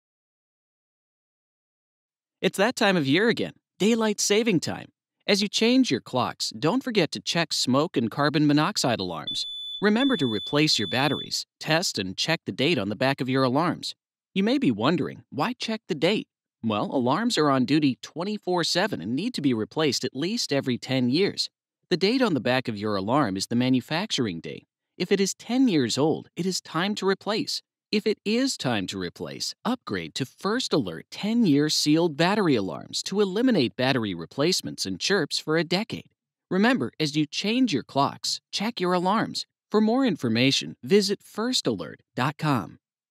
Check Your Alarms During Daylight Saving Time
It’s that time of year again, Daylight Saving Time! As you change your clocks, don’t forget to check your smoke and carbon monoxide alarms. Remember to replace the batteries, test and check the date on the back of your alarms. The date on the back of the alarm is the manufacture date. If it is 10 years old, it is time to replace. If it is time to replace, upgrade to First Alert 10-year sealed battery alarms to eliminate battery replacements and chirps for a decade. Remember as you change your clocks, check your alarms.